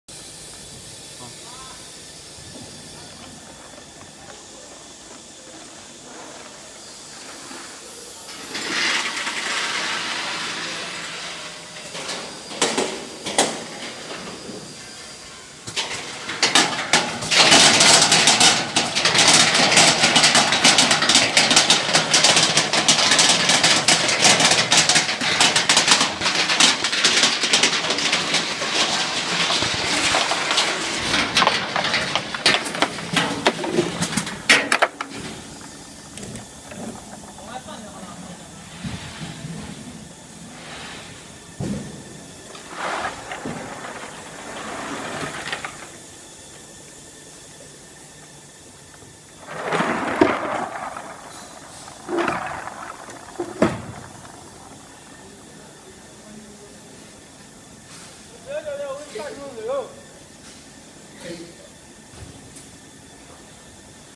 Angelon belt type nuts color sorter special for whole shell walnuts/
High-definition CCD image Sensor for accurate recognition,high-speed data processing,high-quality image processing technology,touch screen display and operating platform,real-time display of actual material image,advanced High-accuracy Valve and Nozzle,well-designed transport system,unique lighting system,lower damage ratio with belt-type structure,high sorting accuracy with double belt-type structure,lower optimized carryover special for nuts.So you can get a higher sorting accuracy,lower damage of nuts and more stable performance.